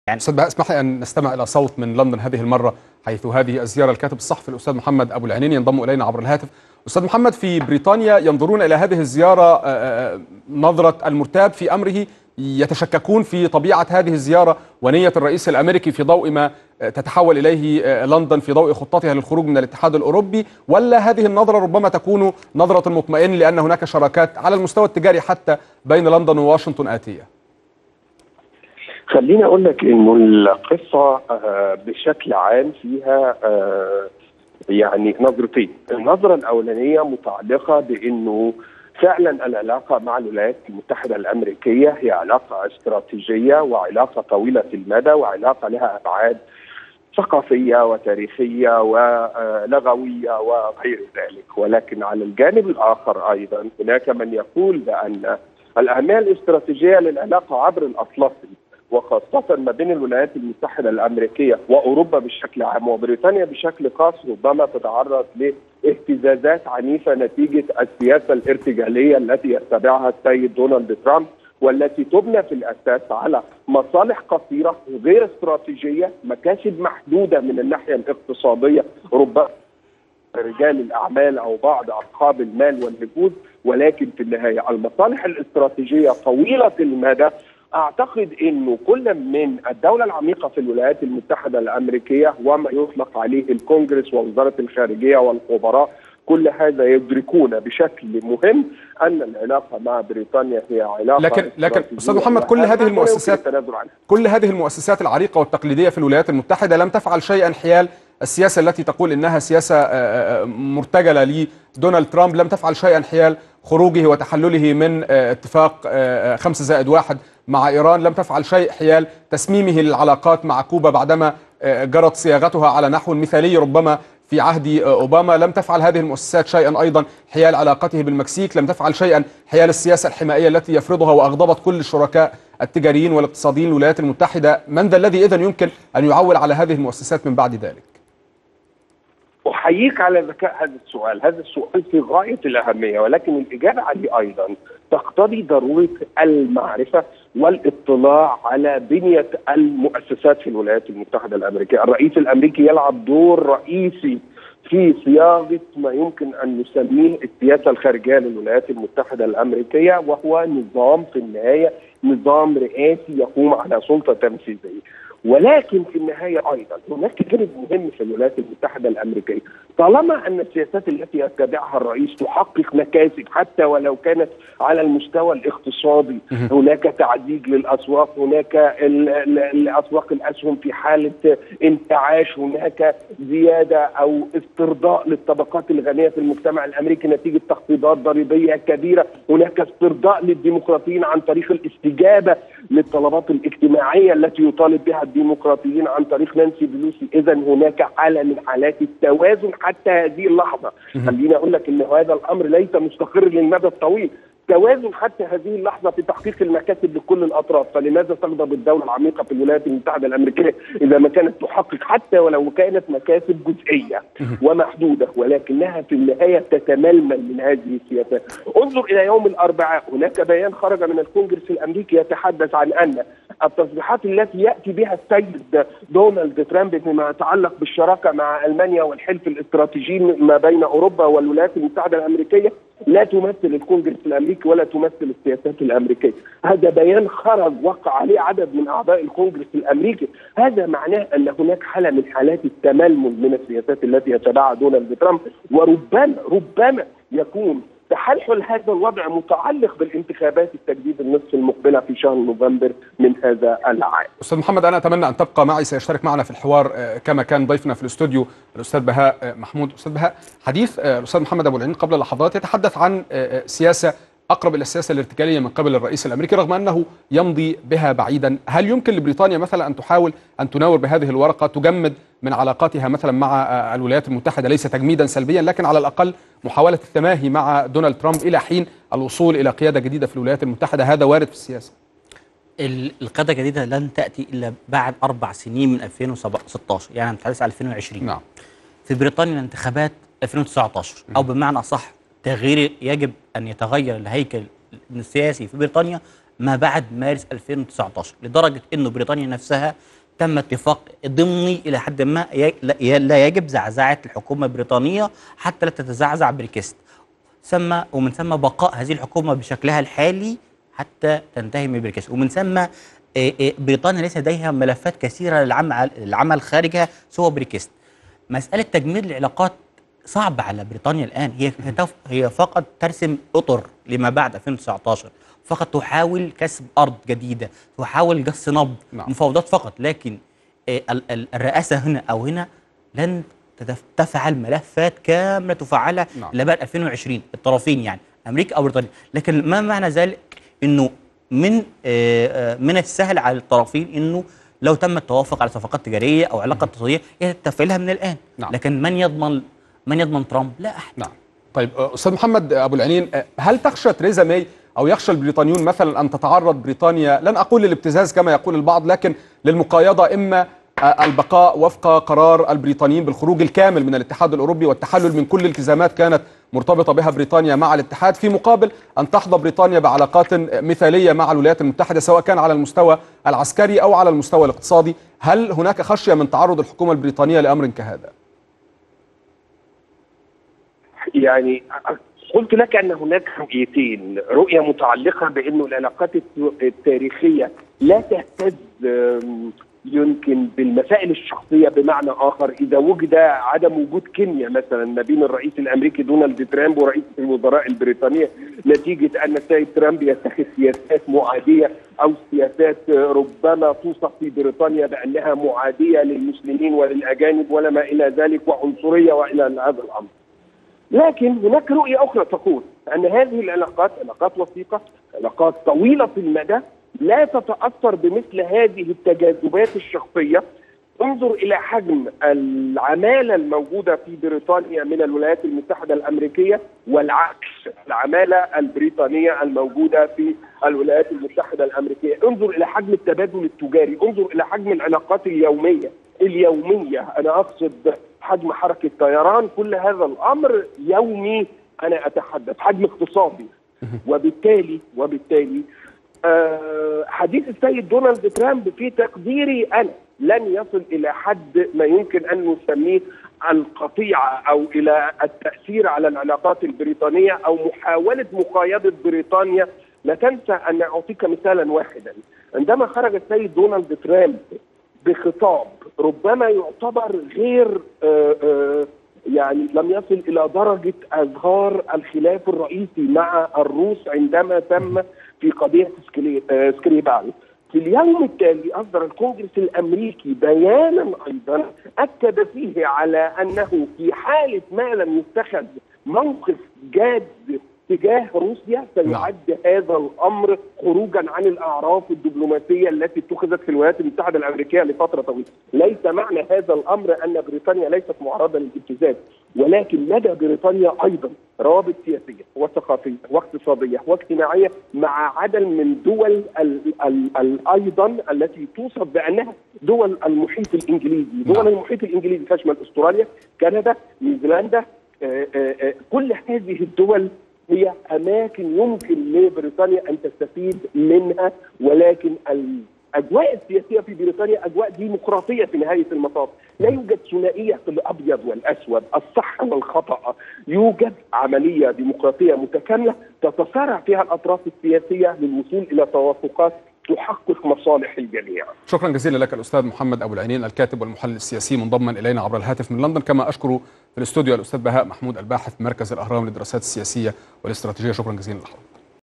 مداخلة محمد أبو العينين حول زيارة الرئيس ترامب لـ بريطانيا لبحث أزمة بريكست
مداخلة محمد أبو العينين كاتب ومحلل سياسي حول زيارة الرئيس ترامب لـ بريطانيا لبحث أزمة بريكست
الموقع الرسمي للقناة  ..